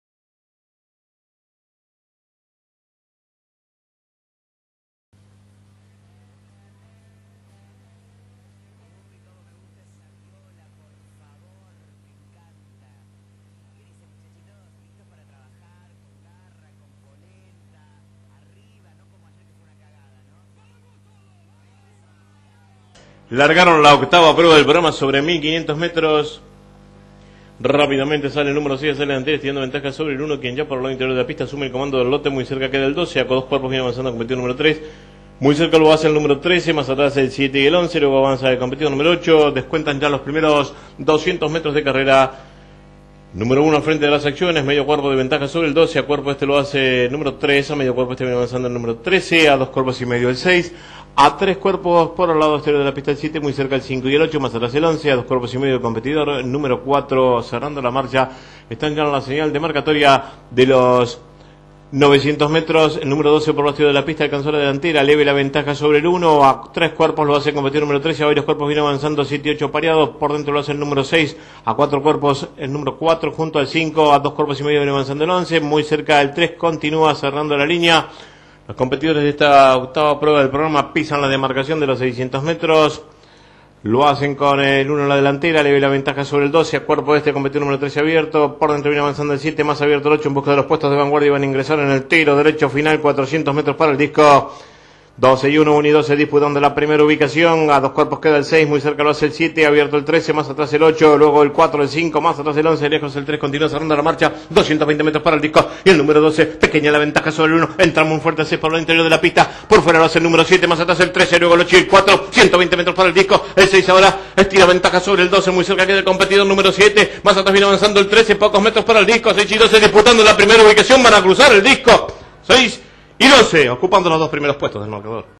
HIPODROMO LA PLATA 01/09/09 8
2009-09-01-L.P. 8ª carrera
1500 metros  1'35''65c
Pista: PH Viento: V/Crz
Premio: ACCIUFFARLA (1997) - (Perdedores)
Premios: $ 15.500, 4.650, 2.480, 1.240 y 930
Pos. Caballo Kg. Jockey # Dist. Pag.
1 MATI TOM 57 A.Barrios  (6)  4,65
2 Musitando 57 M.Fernández  (12) 1/2 cz 2,20
3 Oswald Day 57 L.Alday  (7) 2 cp 10,25
4 San Finder 57 P.Sotelo  (13) 1/2 cp 77,80
5 Es Un Duque 57 G.Hahn  (1) 2 1/2 2,95

Después: 6º Sogyo Sil (4) - 7º Lord Express (8) - 8º Thunder Toss (5) - 9º Potri Loco (11) - 10º Northern Coco (3) -

NO CORR: (2) Rebate Sur, (9) Tan Opinador, (10) Con Nico, (14) Un Festejo y (15) Bock.

Dividendos: (6) MATI TOM $ 4,65, 1,65 y 1,45. (12) Musitando $ 1,45 y 1,25. (7) Oswald Day $ 1,80. Apuesta Exacta (6-12) $ 21,00. Apuesta Imperfecta (6-12) ó (12 -6) $ 11,50. Apuesta Cuatrifecta: (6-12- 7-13) $ 1.772,75
APUESTA DOBLE $ 112,60 -

MATI TOM: 6-MATI TOM: [De 5 años, por SHY TOM (Island Kitty-Blushing Groom) y VICENTA TOSS (Vicente Toss-Asuncion) del stud Hs. Pura India]. Ganó $ 52.230. Corrió 25 veces: 1 primero, 3 segundos, 4 terceros, 2 cuartos, 4 quintos, 11 No Placé.

Entrenador: L.R.Haedo - Criador: Hs. Pura India